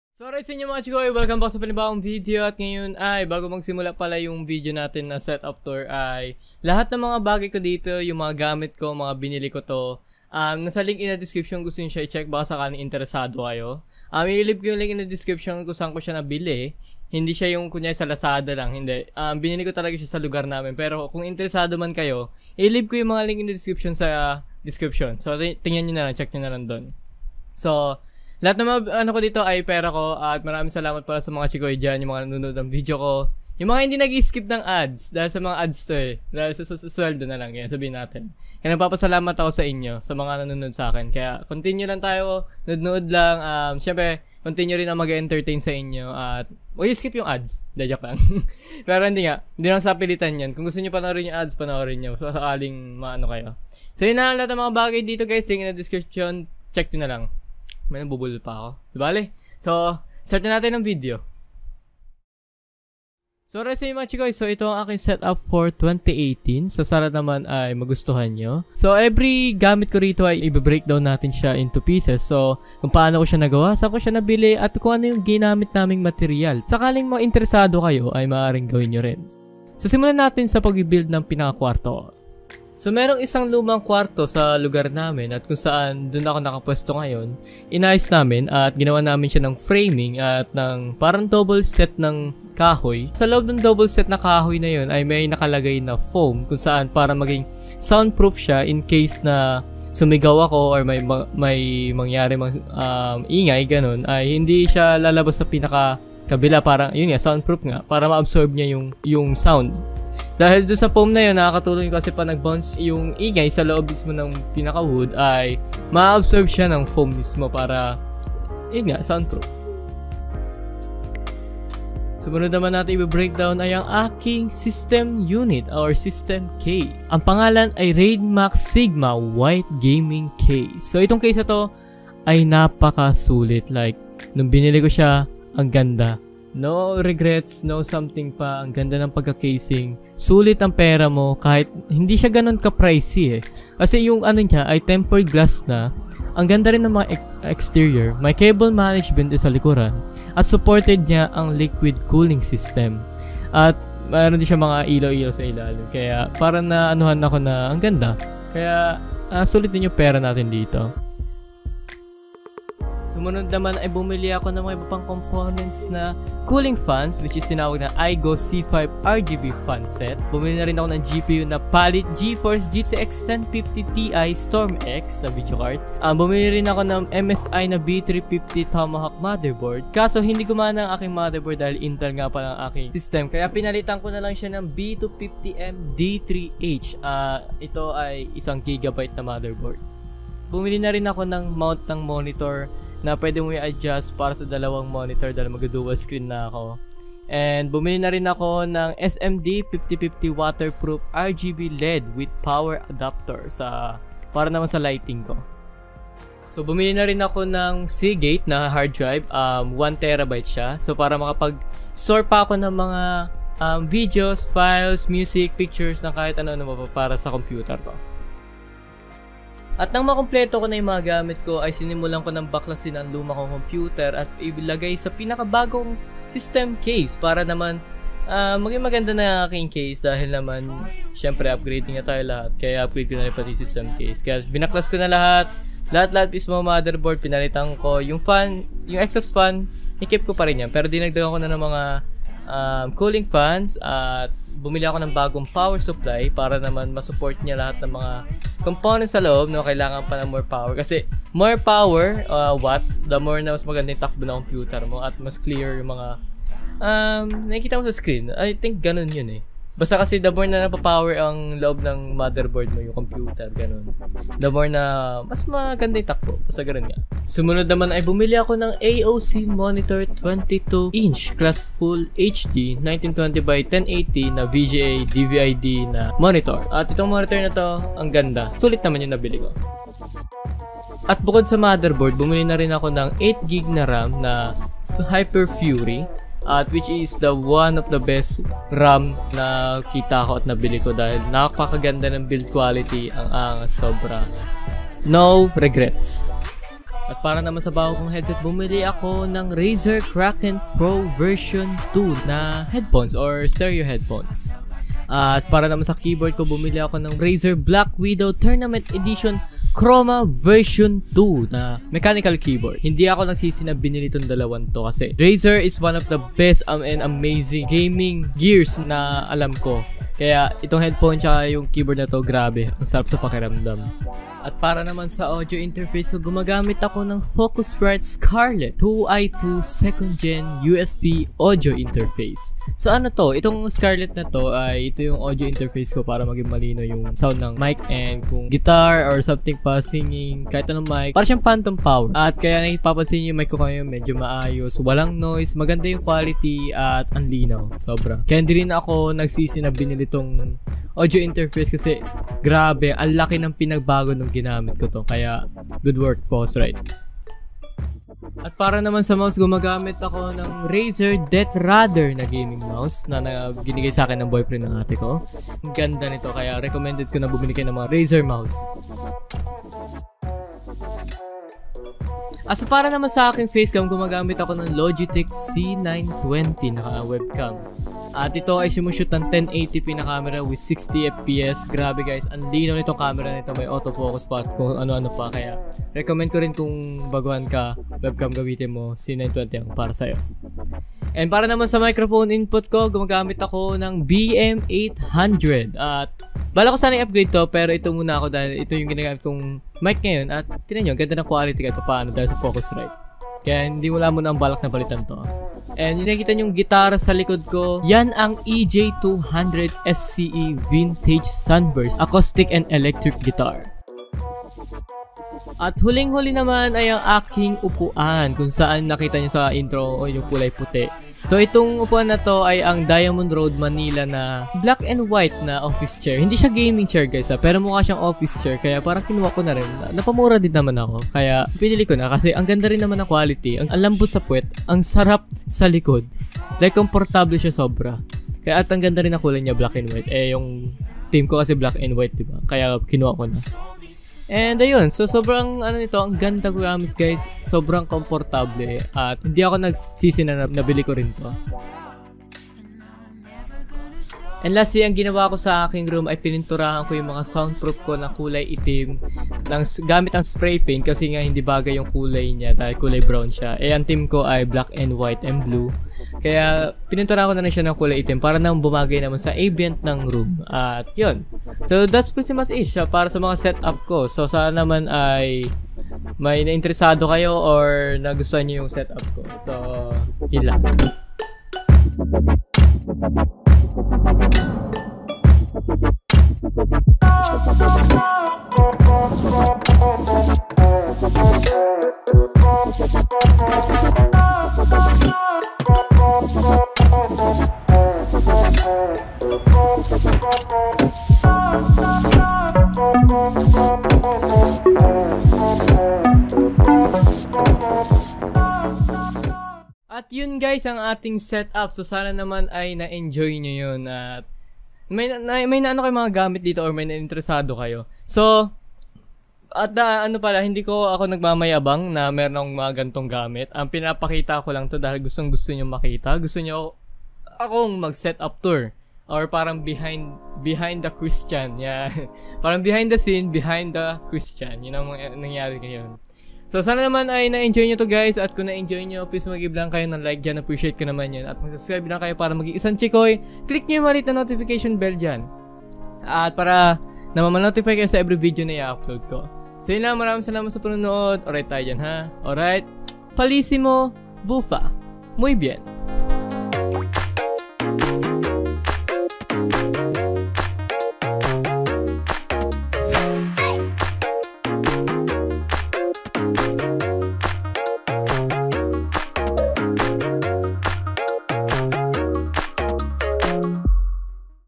MY NEW SETUP (Behind The Kristian) 2018
➡ WELCOME TO MY MAN CAVE!

Chair: No Link :(

PC SPEC:
Intel Core i5-7400 3ghz
GTX 1050 Ti 4gb ddr4
16GB RAM
2x 1 TB Seagate Barracuda HDD

Almost 60K+ Pesos ang nagastos ko
PARA MAGING CHIKOY!
´´´´¶¶¶¶¶¶¶¶¶¶¶  ★彡 ORAYT 彡★